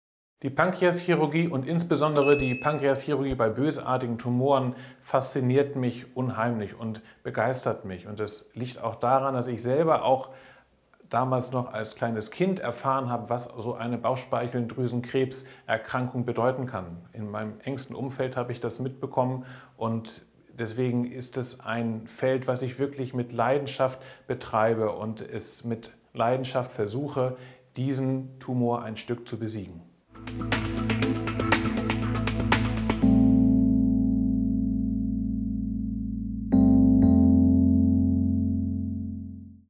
Deshalb fasziniert mich die Pankreaschirurgie! - Besondere Momente | Asklepios
Prof. Dr. Ansgar Michael Chromik hat einen ganz persönlichen Antrieb, um bösartige Tumoren an der Bauchspeicheldrüse zu behandeln. Warum das so ist und er so sehr für seinen Beruf brennt, erklärt der Chefarzt für Allgemein- und Viszeralchirurgie und Pankreaschirurg im Interdisziplinären Pankreaszentrum am Asklepios Klinikum Harburg in diesem Video.

Übrigens: Das Asklepios Klinikum Harburg ist als Kompetenzzentrum für Pankreaschirurgie zertifiziert!